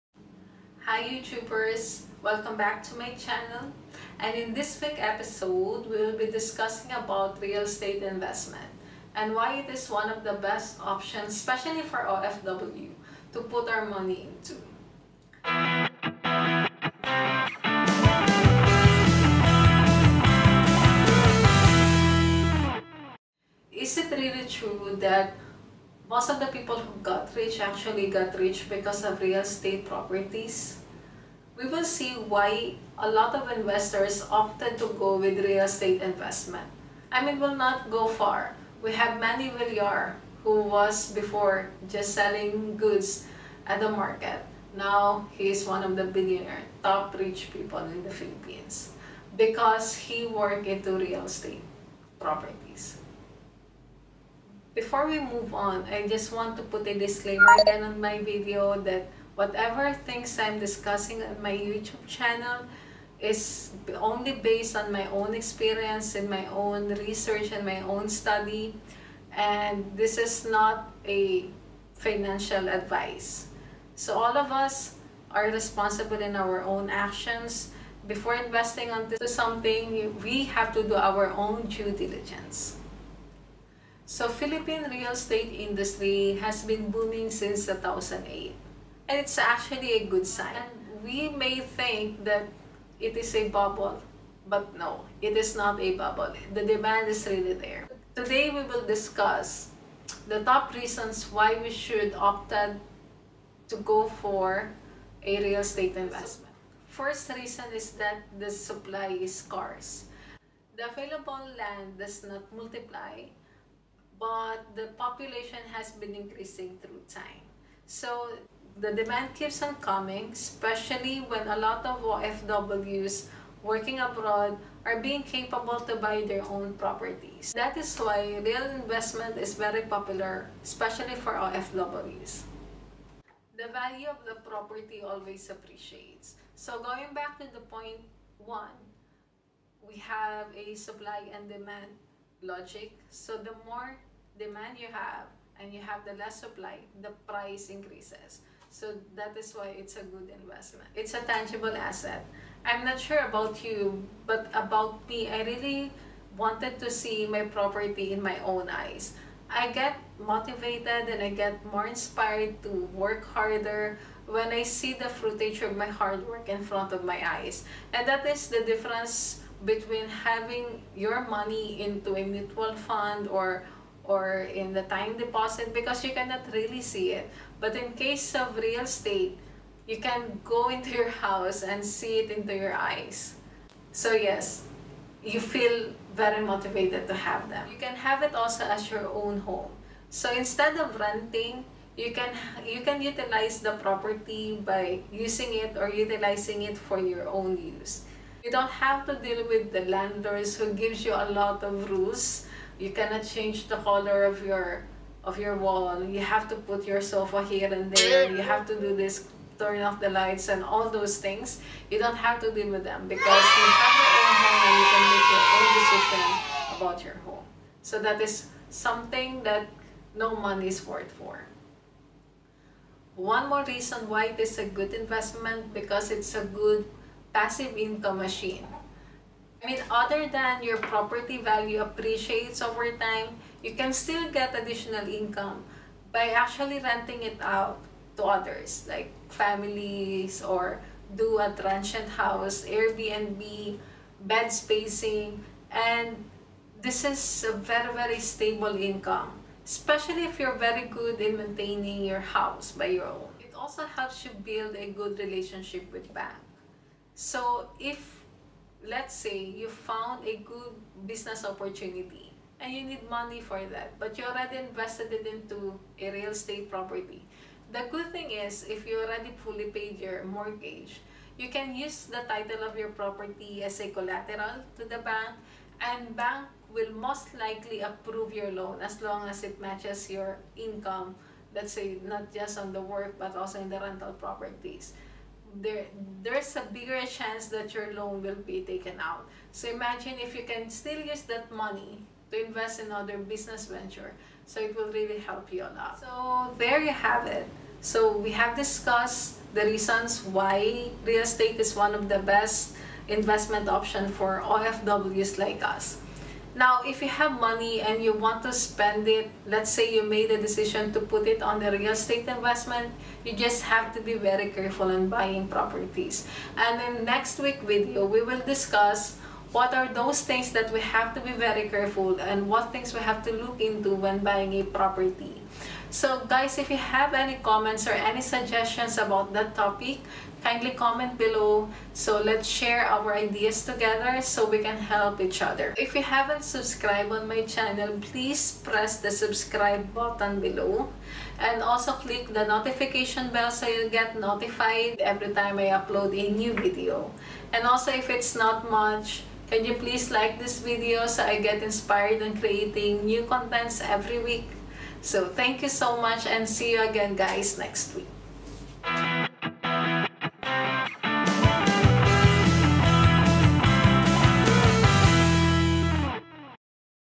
Financial Vlog#03 - Is real estate investment a good option???
In this video we will be discussing on why real estate investment is something that OFWs should be considering.

Let's tackle the major reason why it is a good option for us.